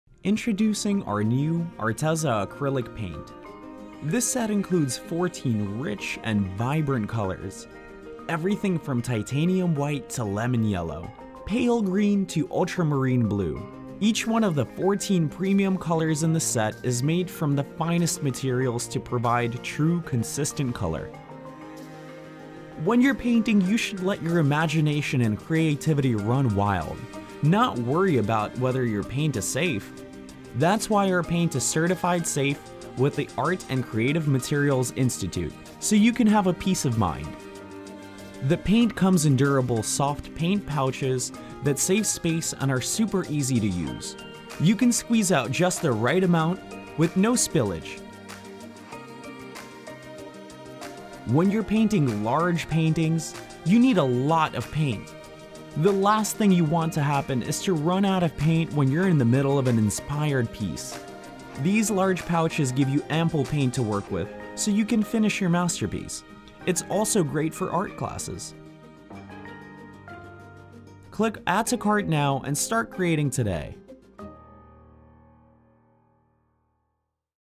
ARTEZA Acrylic Premium Artist Paints Set - 14 Colors
In this ARTEZA paint review, we explore our set of 14 Premium Acrylic Colors in this art video. These golden acrylics will have you throwing out your cheap acrylic paint as you watch this art supply review to buy our professional acrylic paint set! We show you how you'll never run out of paint for intense art sessions during this acrylic paint review. Don’t be restricted by limited art supplies--let your imagination run unbounded and free.

▬▬ ALSO, READ ▬▬

We hope you enjoyed this video and that you learned something new!